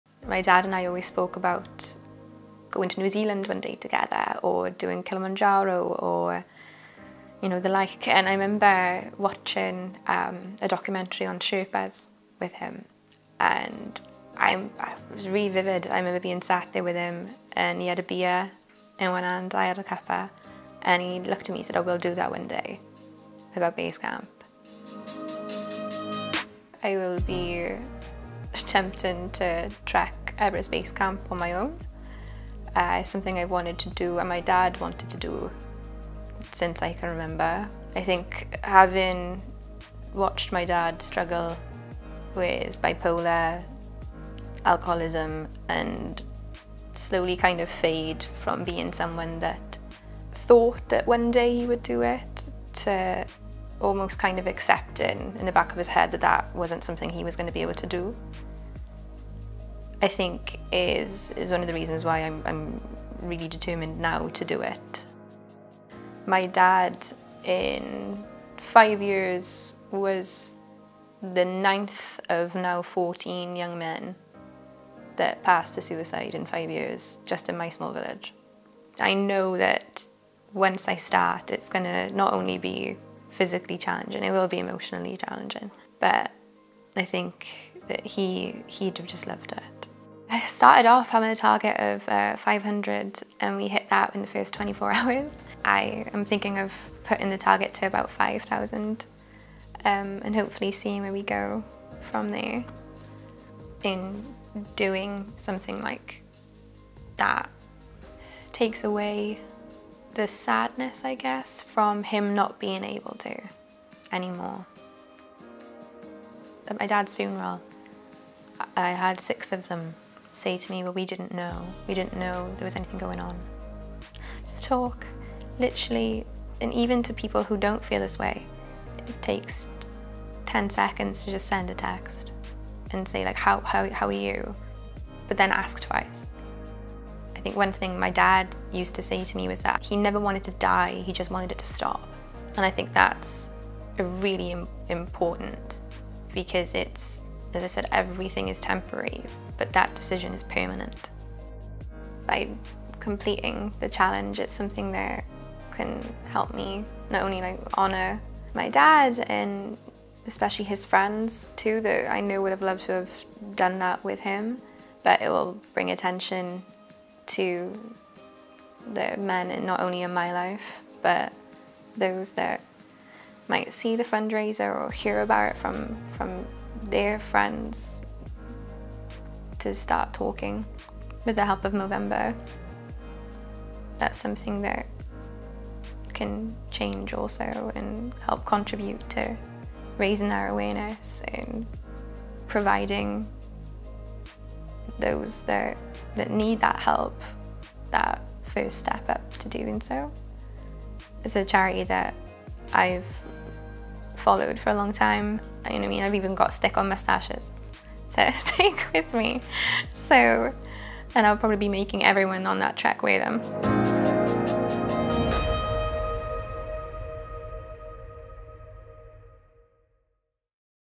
Why I Mo: Beth Jones
Having watched her dad struggle with mental health challenges, Beth lost her father to suicide 4 years ago.

“He wasn't only an extraordinary dad, but a person, too. My dad was not afraid of challenges, and often sought them out. Though the biggest challenge that my dad faced was bipolar.”

They had always spoken about going to New Zealand one day together or doing Kilimanjaro. But after watching a documentary on Sherpas, they had agreed one day to climb Base Camp.

Next March, Beth is going to trek to Everest Base Camp on her own in her dad's memory.